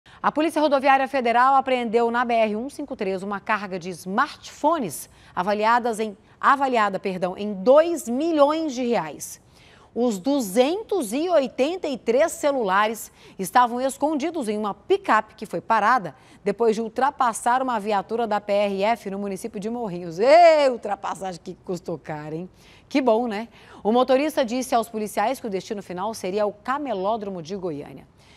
283 CELULARES: POLÍCIA APREENDE NA BR-153 UMA CARGA DE SMARTPHONES AVALIADA EM DOIS MILHÕES DE REAIS
283 CELULARES: A POLÍCIA RODOVIÁRIA FEDERAL APREENDEU NA BR-153 UMA CARGA DE SMARTPHONES AVALIADA EM DOIS MILHÕES DE REAIS.